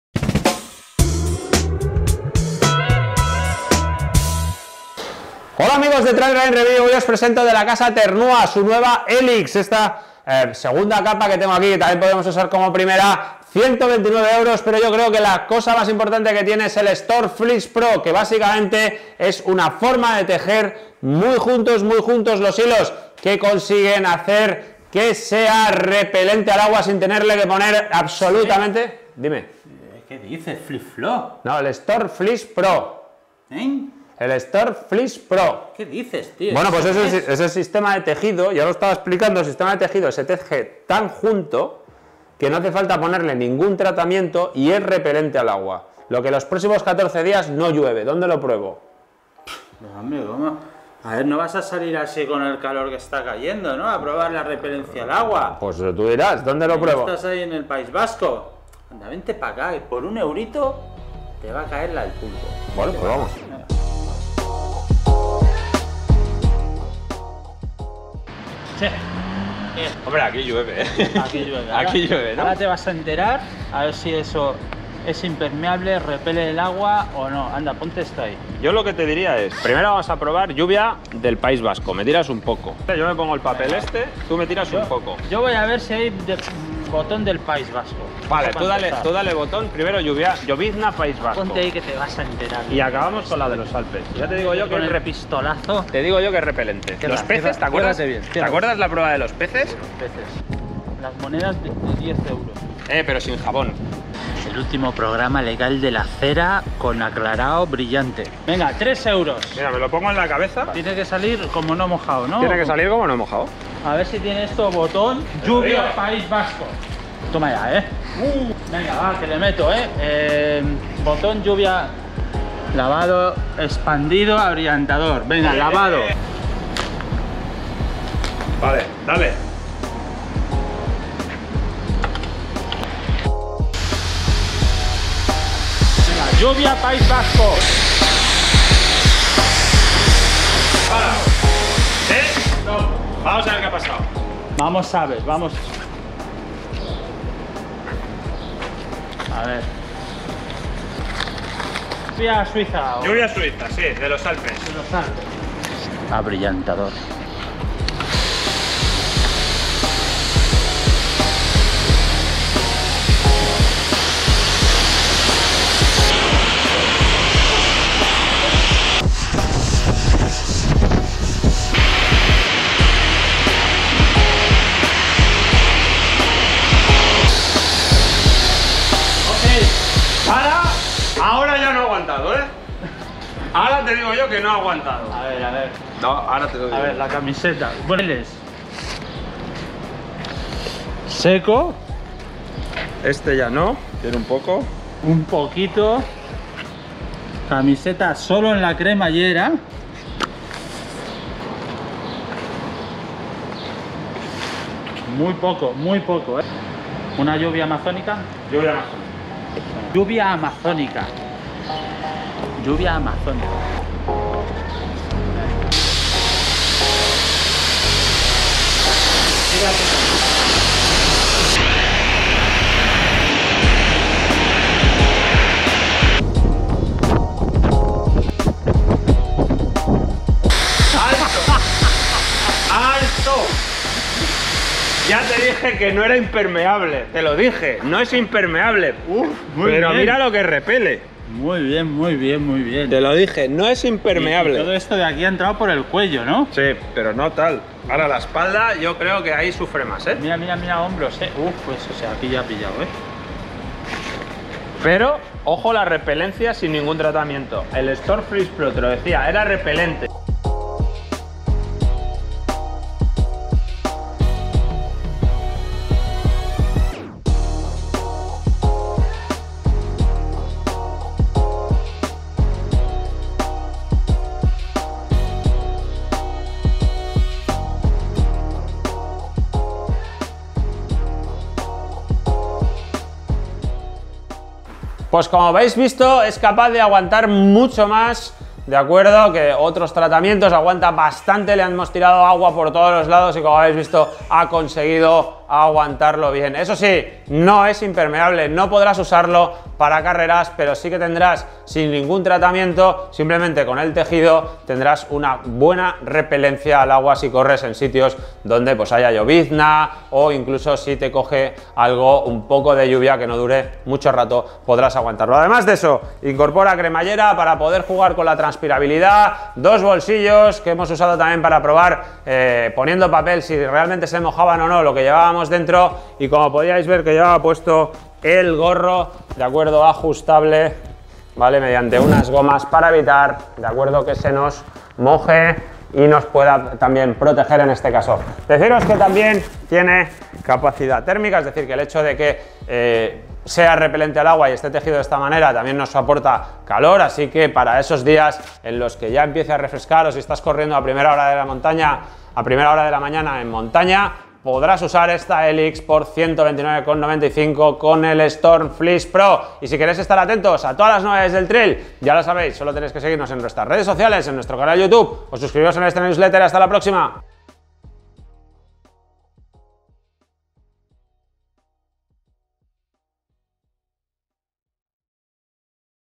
Ternua Helix Jacket Review - La chaqueta casi impermeable sin membrana
Ternua Helix Jacket
La chaqueta Ternua Helix Jacket es una prenda, para utilizar como última capa, que es casi impermeable pero que no cuenta con membrana.
Esta Ternua Helix está fabricada en tejido Stormfleece Pro, de alta resistencia al viento y al agua, y con un acabado DWR libre de PFC. Así pues, contamos con repelencia al agua y un material que va a dificultar la entrada de agua.

Para probar esta chaqueta Ternua Helix nos la hemos llevado a someterla a varios tipos de lluvia (artificial):
- Lluvia del País Vasco
- Lluvia de los Alpes
- Lluvia Amazónica

¿Se habrá mojado la camiseta interior después de esta durísima prueba? ¿Habrá funcionado el tejido Stormfleece Pro de esta Ternua Helix?

TRAILRUNNINGReview